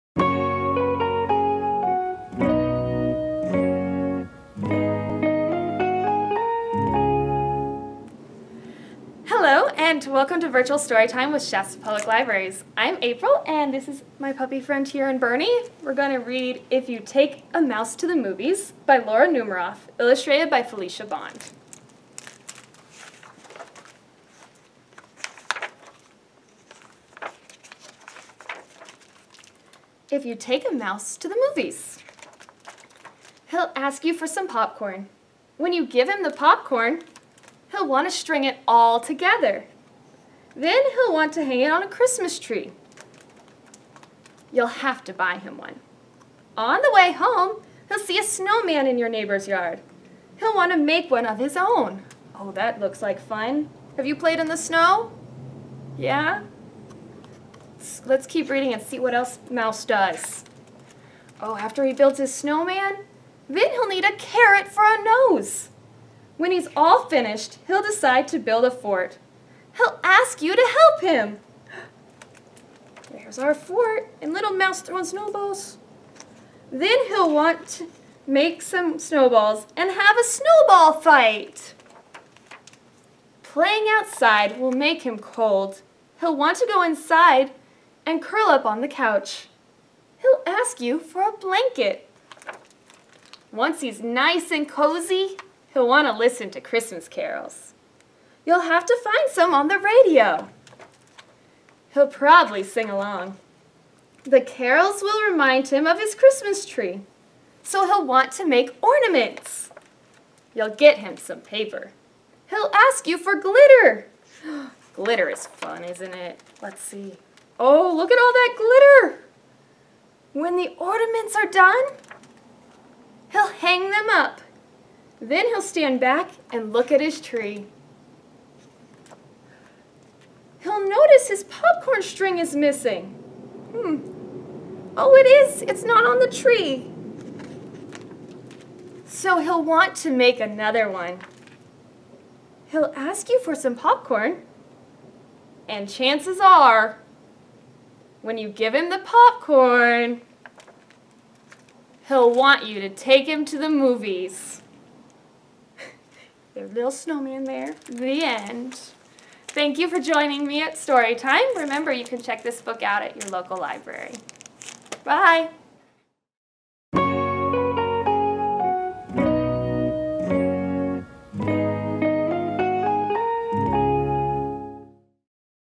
"If You Take a Mouse to the Movies" - Virtual Storytime
What happens when you take a mouse to the movies? Let's find out as we read the book "If You Take a Mouse to the Movies" by Laura Numeroff and illustrated by Felicia Bond. Presented by Shasta Public Libraries in Shasta County California.